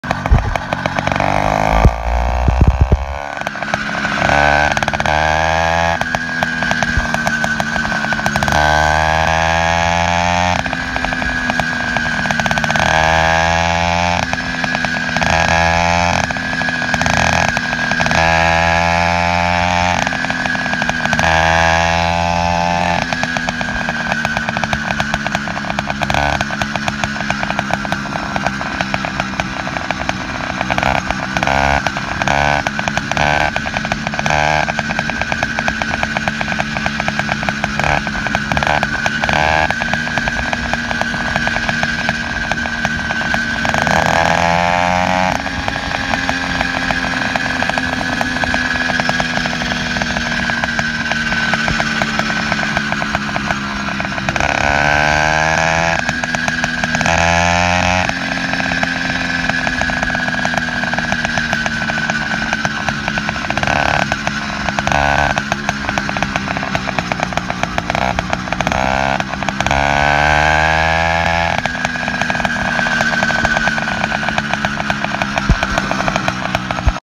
MOTORIZED BIKE ride UP HILL
Quick 1 minute  speed run topped at 30mph not even full gassing it.